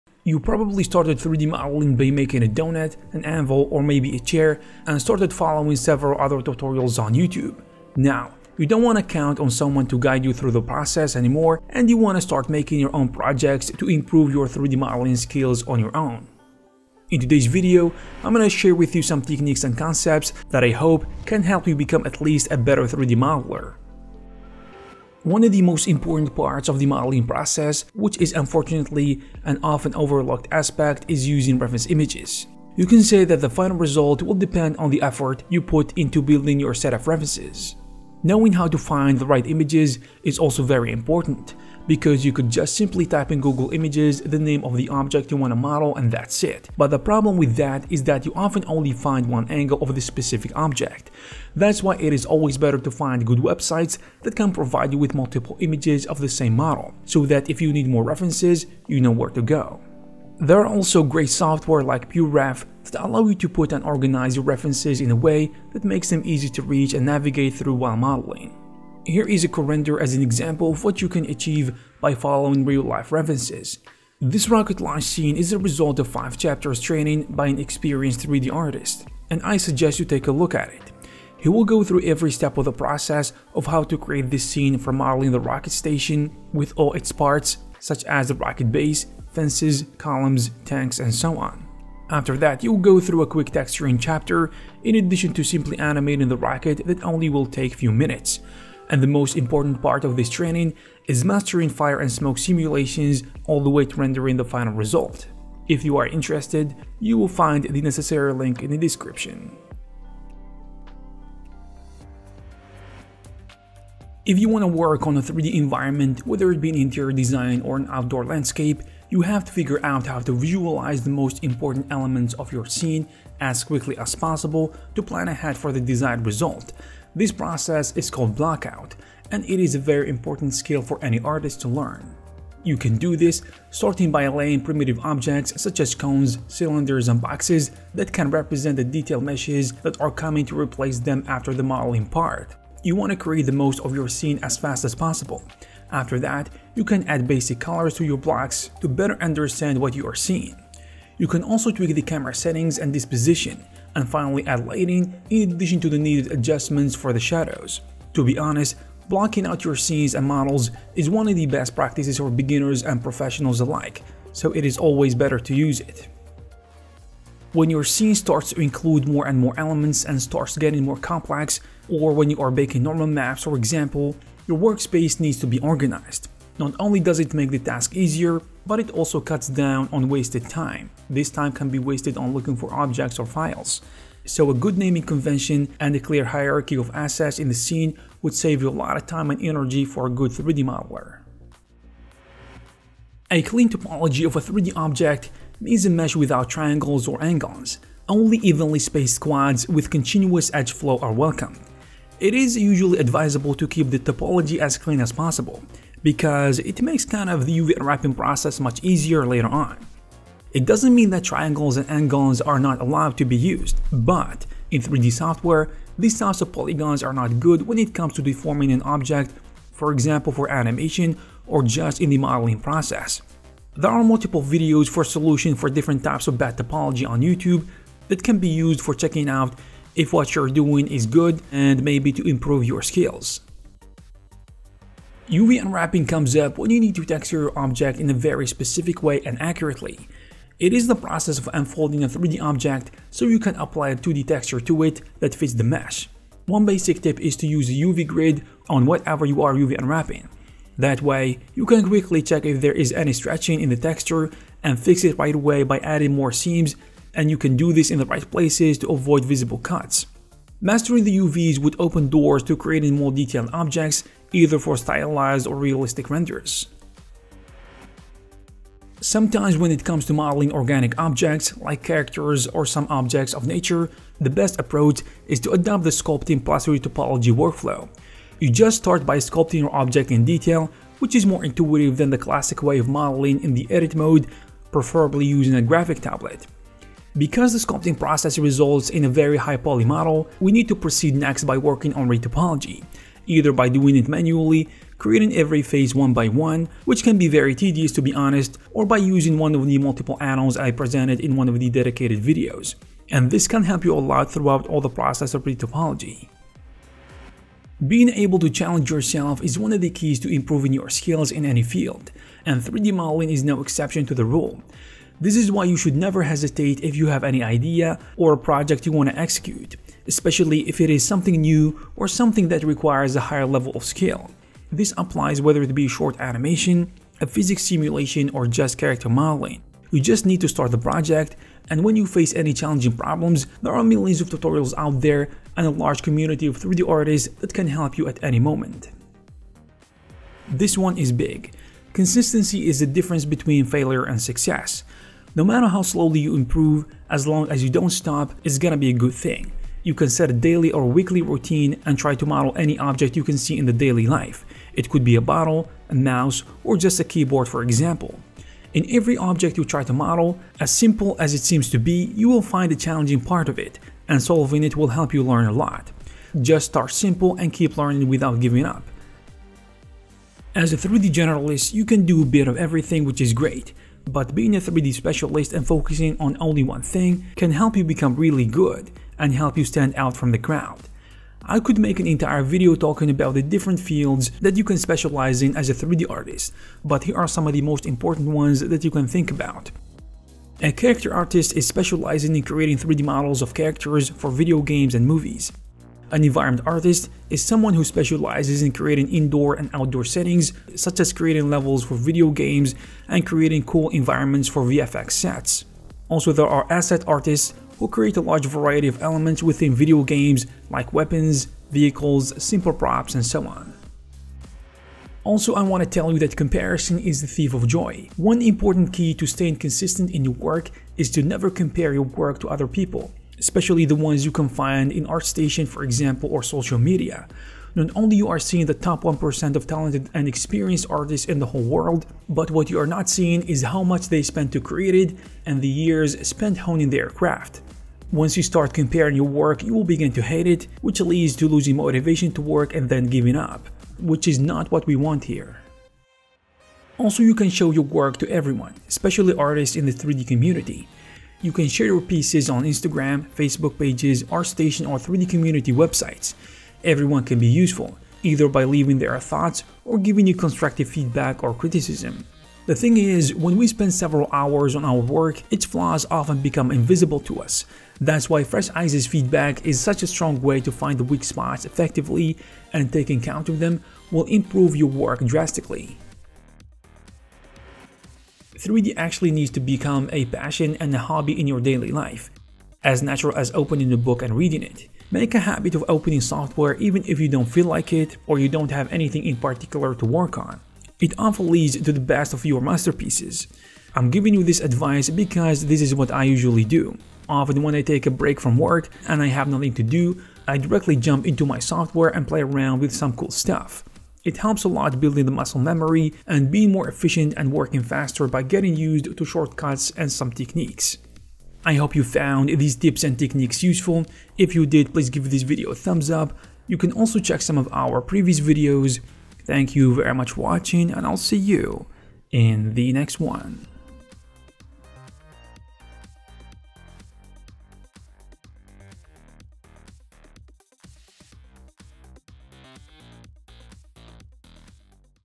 How to improve your 3D modeling skills
In today’s video, I’m going to share with you some techniques and concepts that, I hope, can help you become at least a better 3D modeler.

Animate A Rocket Launch: Smoke & Fire Simulation In Blender